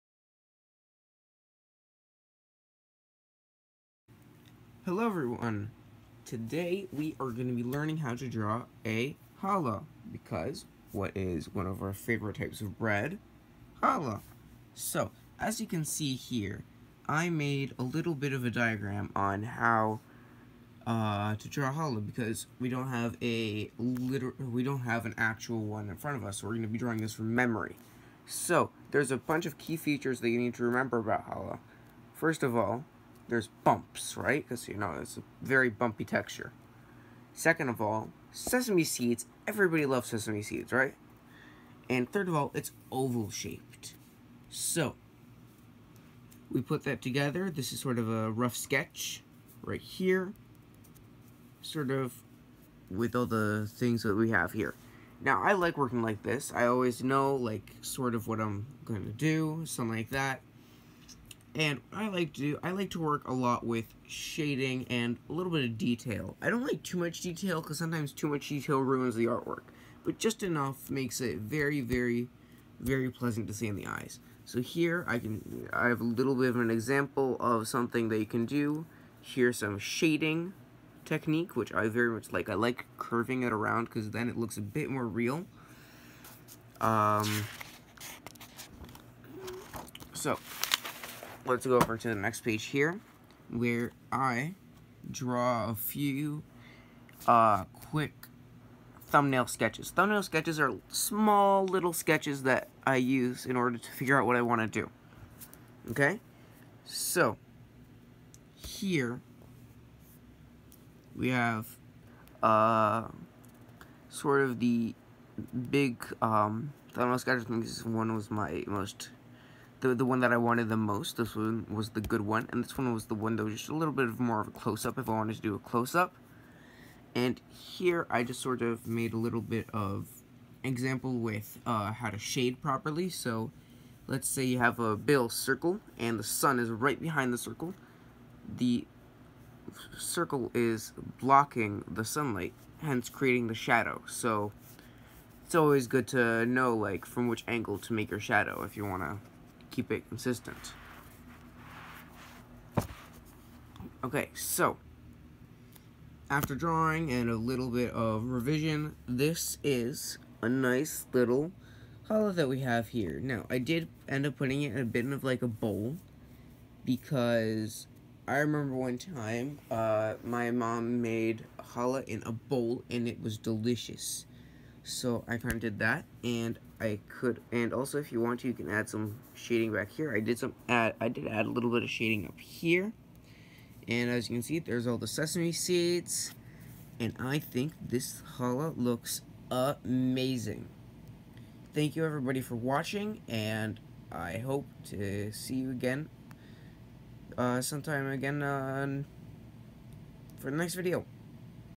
Madrichim Corner - Learn How To Draw A Challah!
Grab a pencil and paper and join our Madrichim for a drawing class. Learn some awesome new art skills, and be ready for Shabbat with your very own  piece of challah art!